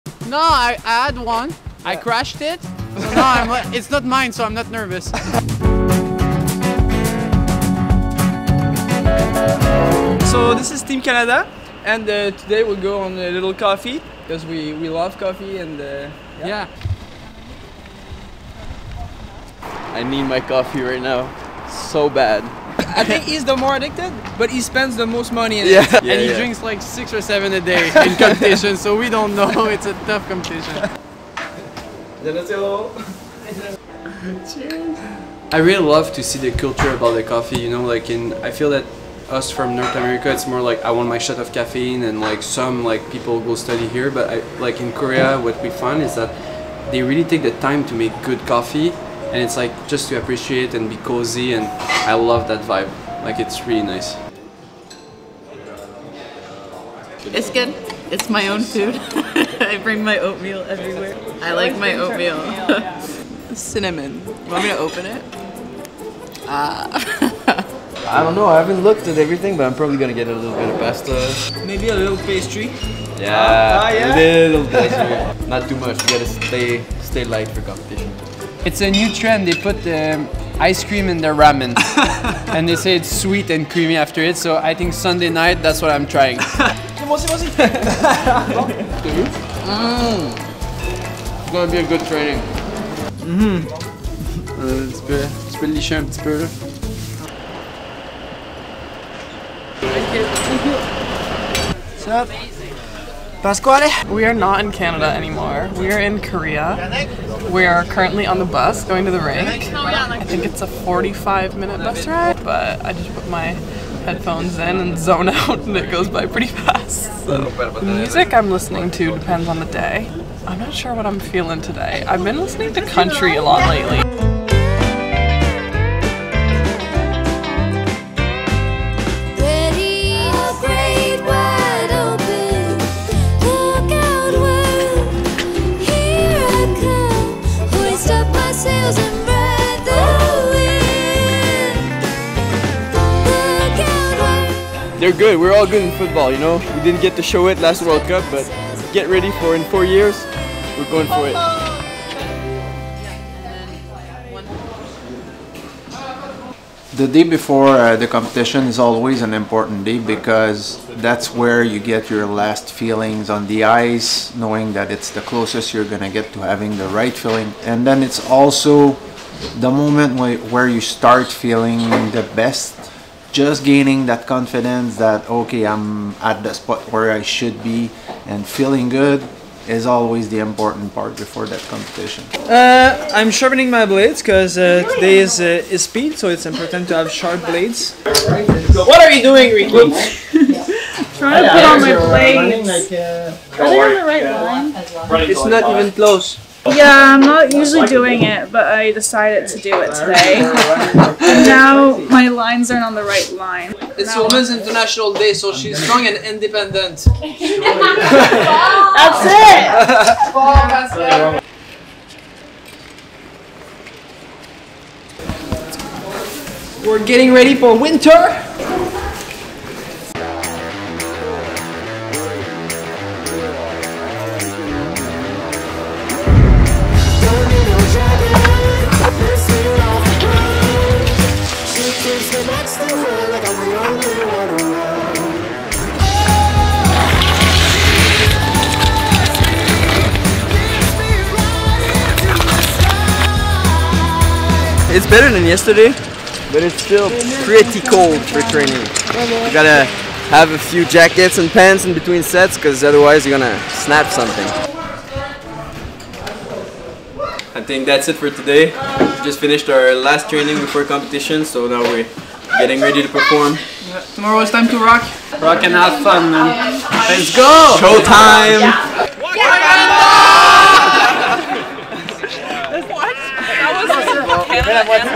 One Day With Team Canada | Seoul 2023
From tasting the best coffees around the hotel in Seoul (KOR) to testing the ice, the Canadian team shows you everything they did leading up to competition day at the KB Financial Group ISU World Short Track Championships!